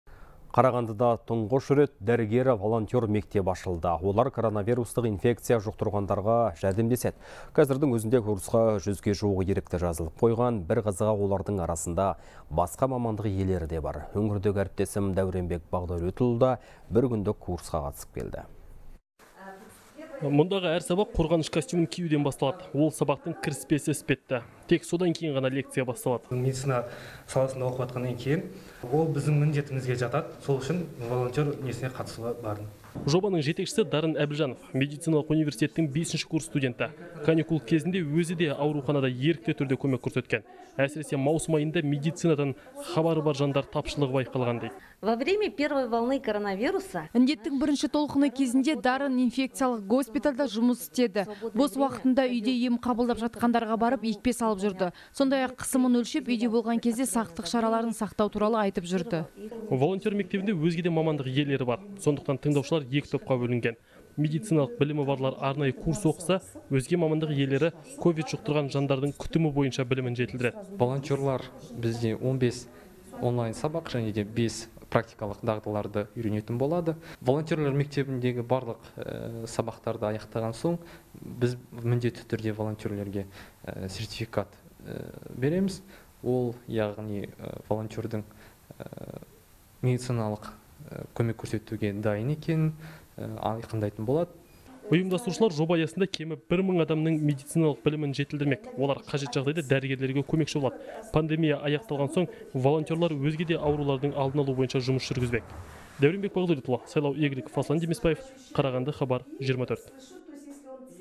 Қарағандыда тұңғыш дәрігер-волонтер мектебі ашылды
Қарағандыда тұңғыш рет дәрігер-волонтер мектебі ашылды, деп хабарлайды «Хабар 24».

Олар коронавирустық инфекция жұқтырғандарға жәрдемдеседі. Қазірдің өзінде курсқа 100-ге жуық ерікті жазылып қойған. Бір қызығы, олардың арасында басқа мамандық иелері де бар. Өңірдегі арна тілшісі Дәуренбек Бағдәулетұлы да бір күндік курсқа қатысып келді.